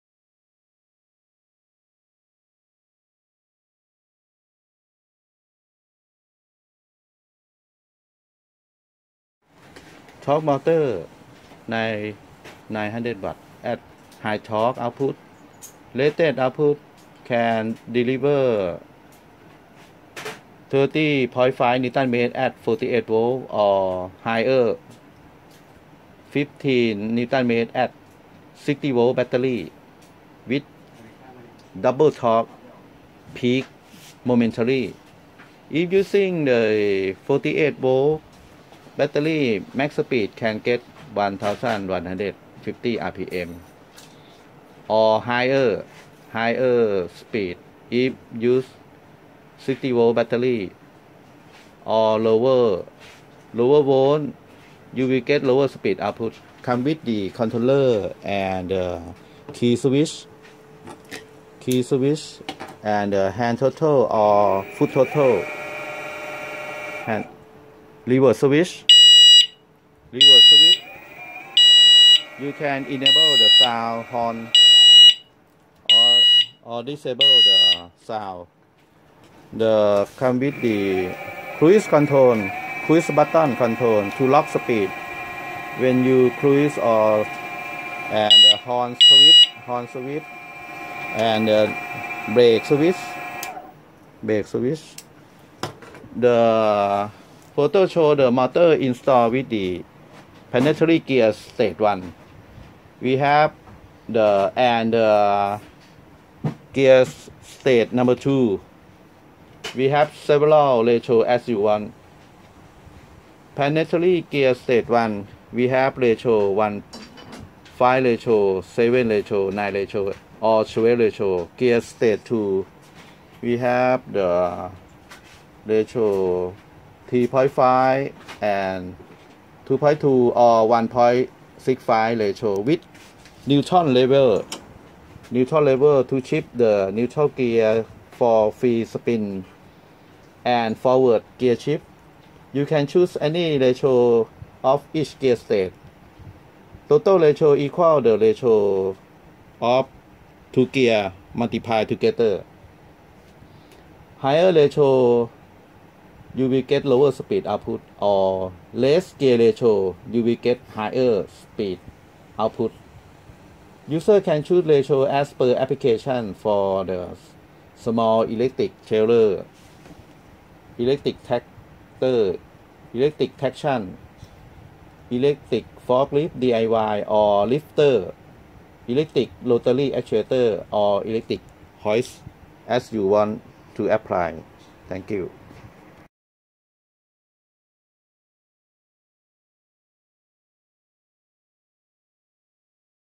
TORQUE MOTOR 900WATT AT HIGH TORQUE OUTPUT
TORQUE MOTOR 900WATT  AT HIGH TORQUE OUTPUT /RATED = 13.5Nm@48V  or 15Nm@60V  ALL THE TIME  WITH PEAK TORQUE AT DOUBLE TORQUE(momentary) , if USING 48Vbattery  max.SPEED = 1150 RPM  or 60V battery will get max. speed 1350 rpm.  Lower volt =lower speed output !
Come with bldc controller 1000watt  with  function   of  hand or foot Throttle , LED battery indicator,Key switch ,brake switch ,Forward&Reverse switch , Cruise switch(lock speed), Neutral Lever(at Gear stage#2) to shift to Neutral Gear for Free-spin .
PHOTO SHOW   MOTOR INSTALLED WITH PLANETARY GEAR  RATIO 1:9  AS GEAR STAGE#1
AND
ratio 1: 3.5  at Gear Stage#2    as  Calculation for  OutPut TORQUE in details
PlanetatryGear Stage #1 , We have several Ratio as = 1:5,  1:7, 1:9 , 1:12
Gear Stage #2  , have high strength and High permission torque more than 350Nm., We have  selectable  Ratio  = 1:3.5 , 1:2.2  , 1:1.65    Neutral Lever(at Gear stage#2) to shift to Neutral Gear for Free-spin and  Forward gear shift .
user  can choose any Ratio of each Gear Stage , the total ratio =equal ratio stage #1 multiply  ratio stage#2 ,
Higher gear ratio ,  lower output speed  or Less Gear ratio ,higher  speed output van  get!
user can choose  ratio as per applications  such as small electric-Trailer , small electric-Tractor ,e-Traction  ,electric-Forklifts DIY , e-Lifter , electric-rotary Actuator  Electric-Hoist